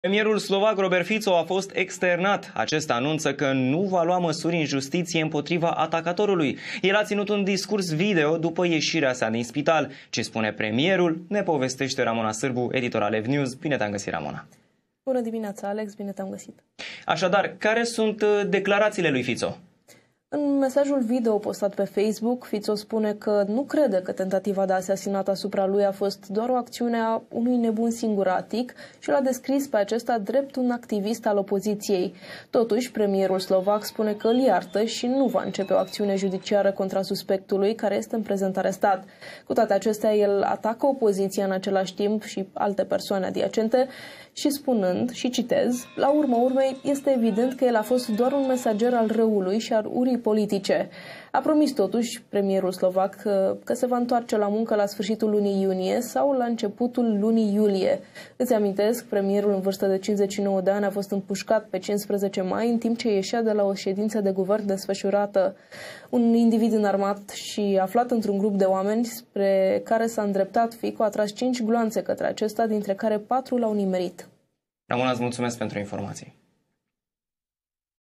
ACUM: Robert Fico a fost externat. Premierul anunță că „îl iartă” pe atacator
➥ Premierul slovac anunță că nu va lua măsuri în justiție împotriva atacatorului
➥ Fico nu crede că tentativa de asasinat a fost doar o acțiune a unui „nebun singuratic”
➥ Premierul slovac promite că se va întoarce la muncă la începutul lunii iulie